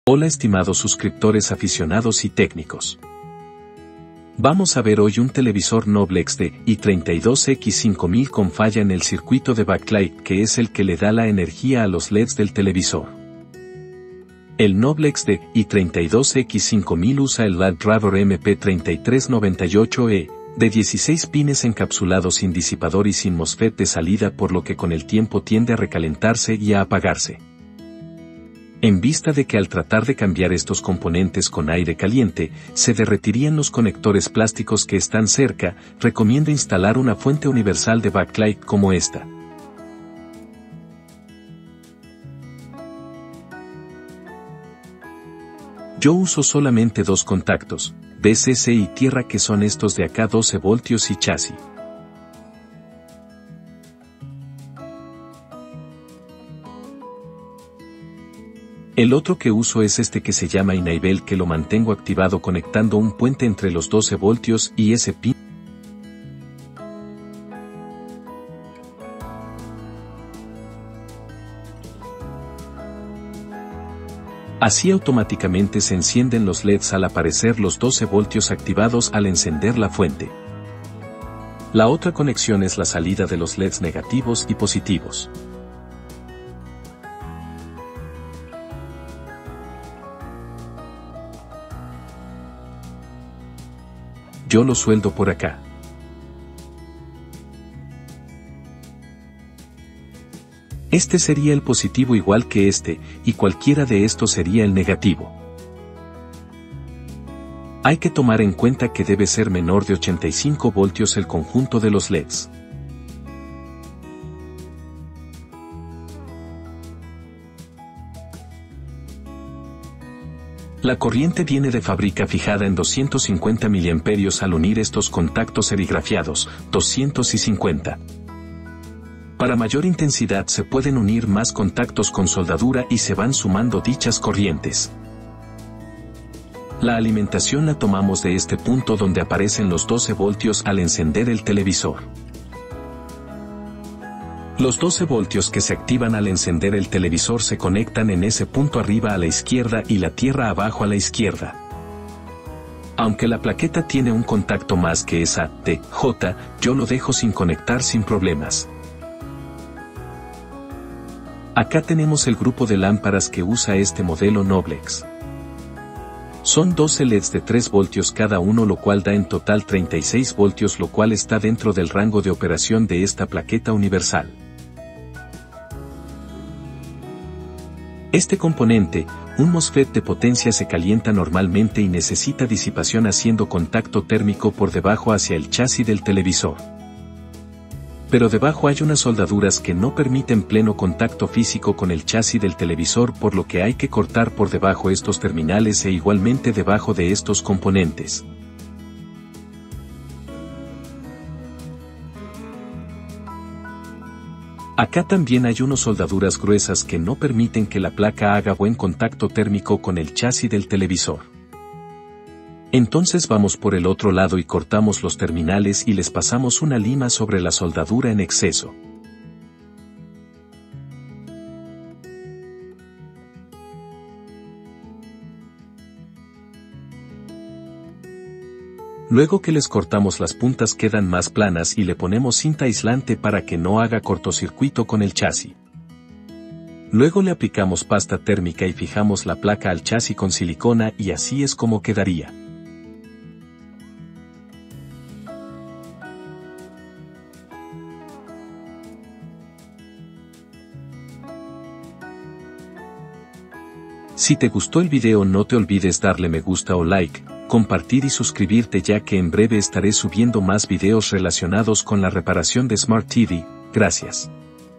Cómo adaptar fuente Universal de retroiluminación en Smart Tv (Backlight)
00:00:00 Introduccion
00:00:45 Identificacion de conexiones de entrada: VCC (12v), ENA y GND
00:01:15 Uniendo VCC con ENA se simplifican las conexiones a sólo dos cables.
00:01:40 Identificacion de conexiones de salida: LED+ y LED-
00:01:51 El voltaje total del grupo de leds en serie no debe ser mayor de 85 voltios. Para voltajes mayores a 85v. re-conectar los leds en serie-paralelo de tal modo que el total dé menos de 85v. como se verá en futuros videos de mi canal.
00:02:00 Ajuste de la corriente de salida. Ya está fijada a 250 ma de fábrica con dos puentes en R13 y R14 abajo a la izquierda. Para corrientes mayores, añadir puntos de soldadura debajo de J3 a J7.
00:02:17 Donde conectar el VCC y el ENA: Buscar un punto de la fuente de 12v que aparezcan al encender el Tv. Como el encendido o "Enable" está puenteado junto con los 12v, los leds encenderán al encender el Tv.
00:02:38 La entrada ADJ o "Adjust" no la conecto para simplificar la instalación ya que no es indispensable. Si alguien desea puede conectarla a la salida PMW "Pulse Width Modulator" o modulador por ancho de pulso que permite al procesador ajustar la luz de fondo por el menu.
00:03:00 Importancia de disipar el calor del Mosfet de potencia. Sobre todo con corrientes mayores de 250ma.
00:03:36 Cortar y limar el exceso de soldadura y alambres en el otro lado de la placa para  aislarlos con cinta y evitar que hagan corto circuito con el chassis del Tv.
00:03:53 Colocar cinta aislante en los contactos cortados y limados.
00:03:58 Añadir pasta térmica debajo del Mosfet de potencia y zonas cercanas para una disipación de calor óptima.
00:04:03 Fijar la plaqueta al chassis del Tv con silicona líquida.

Nota: A veces es mas conveniente instalar una placa fuente universal backlight led como la mostrada en este video que solo cuesta unos 10 dólares, en lugar de tratar de reparar el circuito original que por lo general los ubican muy cerca de conectores plásticos que se afectan con el calor del cautín o desoldador de aire caliente o  tienen diseño de poca disipación y poca protección contra sobrecargas y picos de voltaje que los expone a quemarse de nuevo.